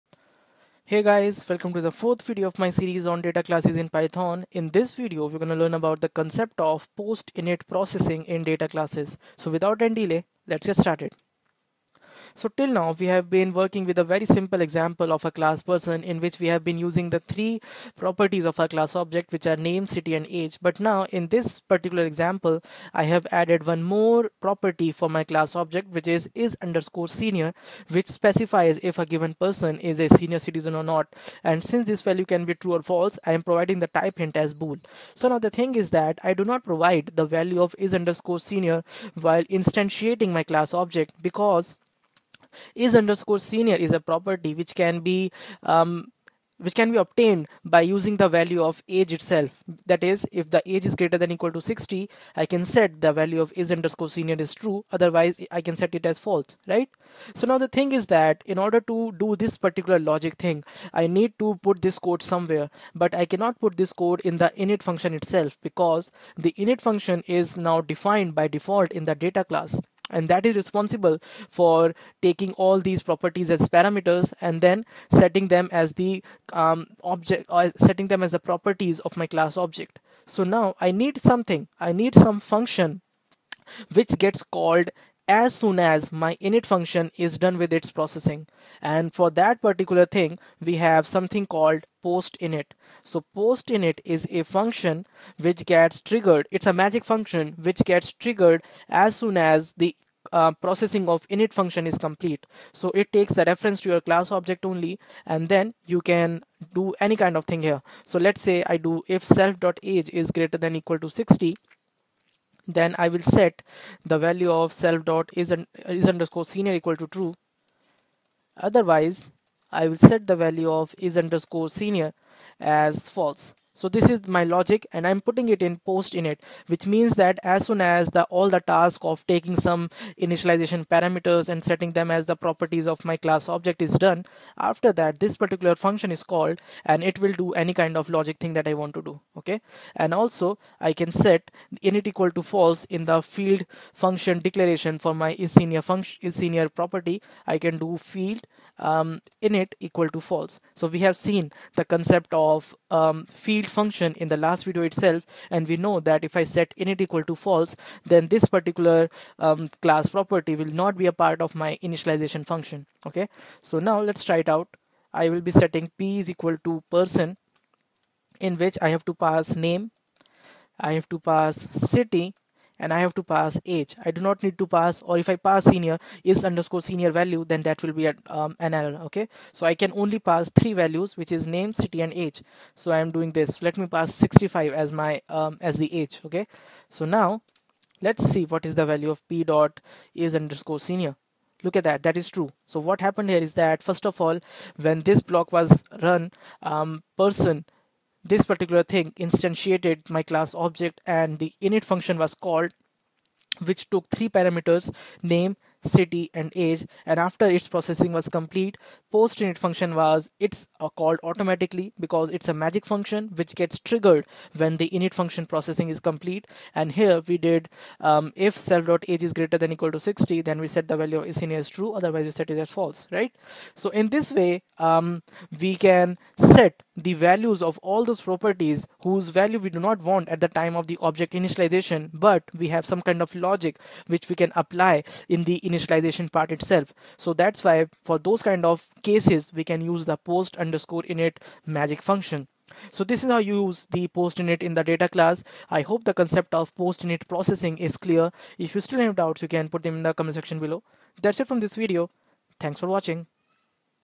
Dataclasses in Python (Part-4) | post-init processing
This video explains the concept of post-init processing in dataclasses.